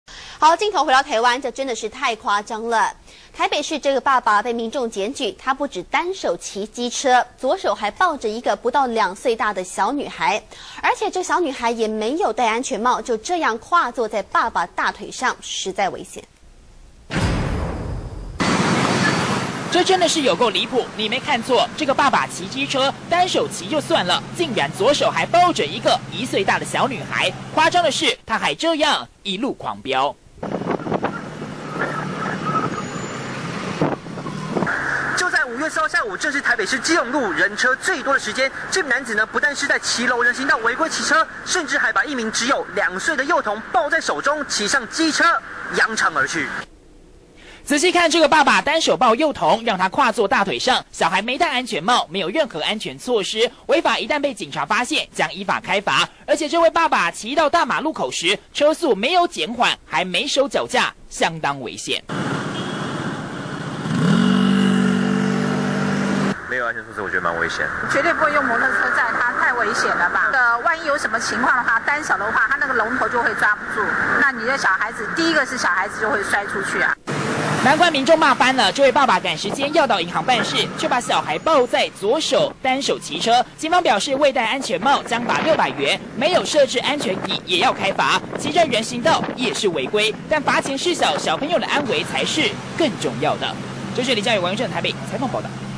險象環生！爸爸單手騎車 左抱1歲多女娃
中天新聞》台北市有一名爸爸遭民眾檢舉，原來他不只單手騎機車，左手還抱著一個不到2歲大的小女孩，而且這位小朋友完全沒有戴安全帽，就這樣跨坐在爸爸大腿上，目擊民眾痛批實在太危險了。